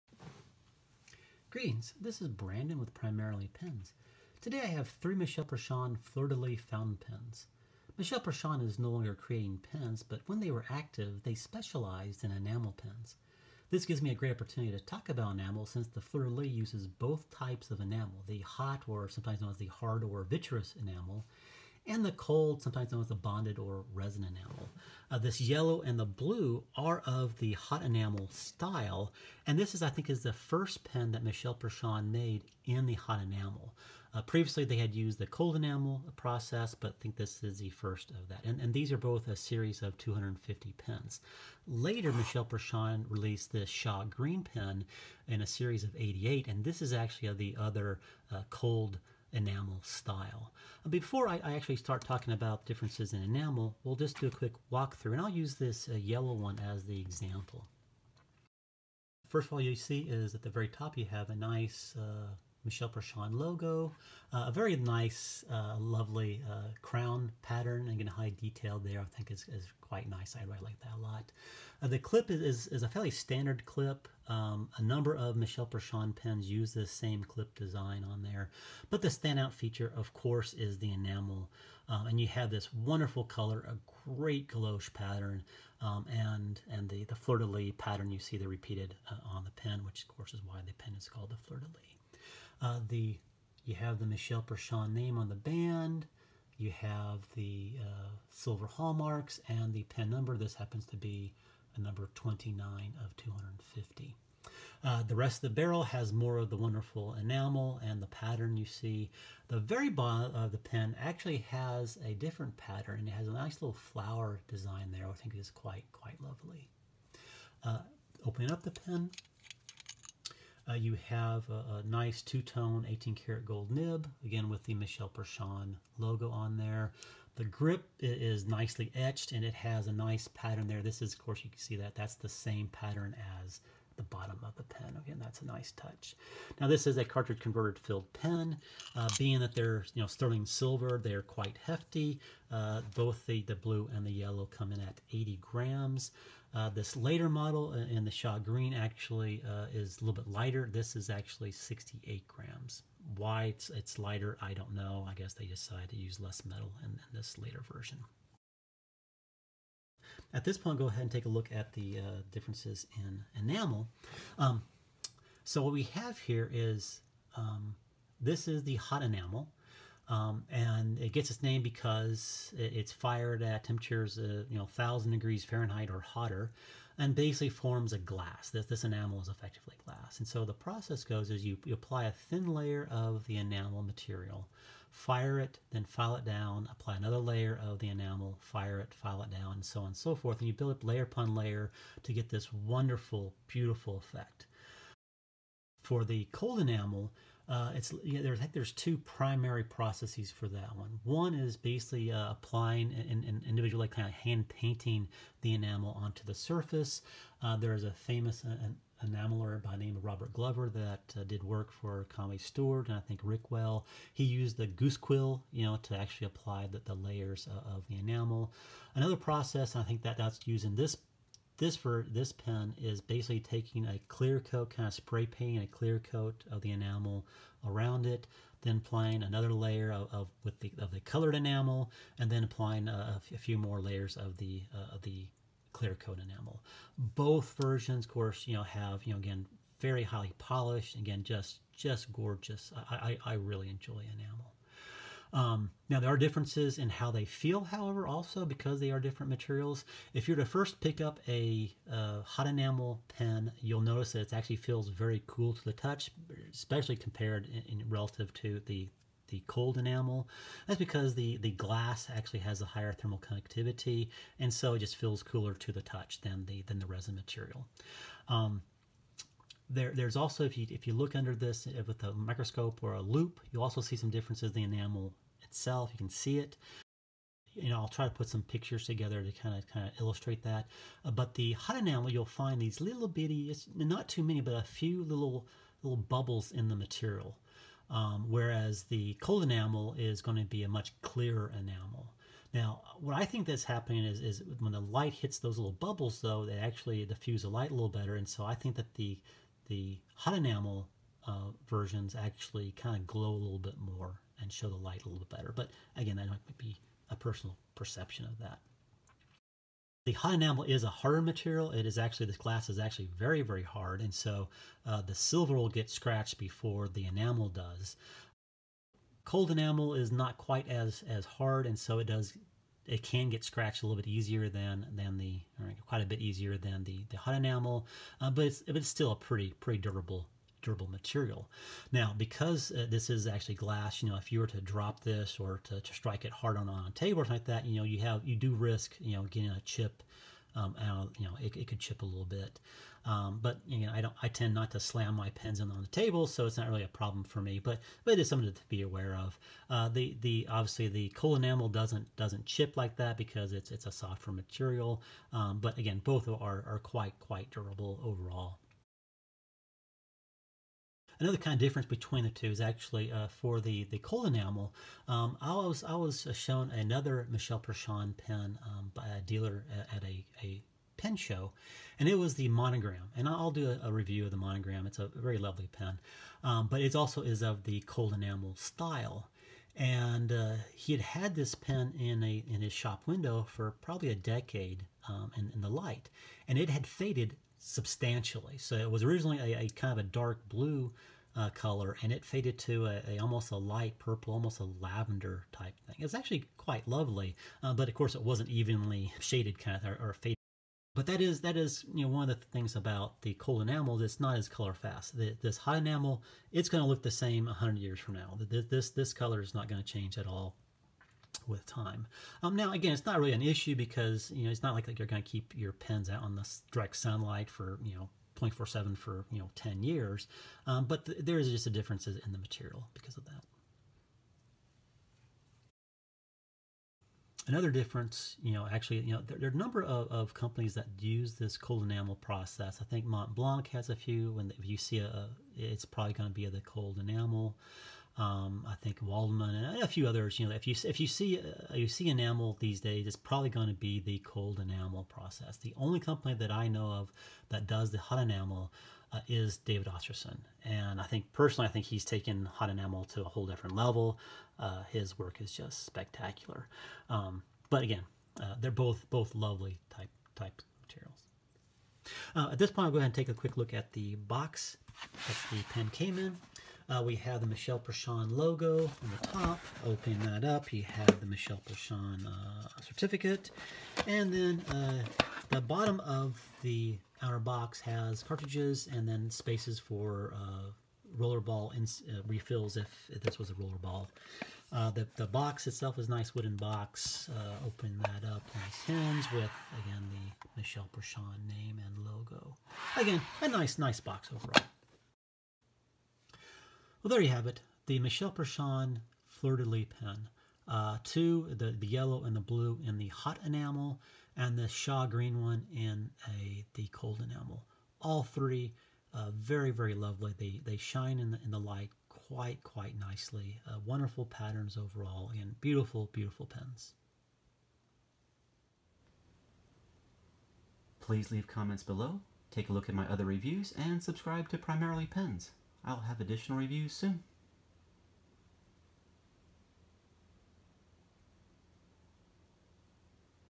Michel Perchin - Fleur de Lis fountain pen and the differences between hot and bonded enamel
Walk through of the Michel Perchin Fleur de Lis fountain pen with a discussion of the differences between  hot (vitreous) enamel and cold (bonded/resin) enamel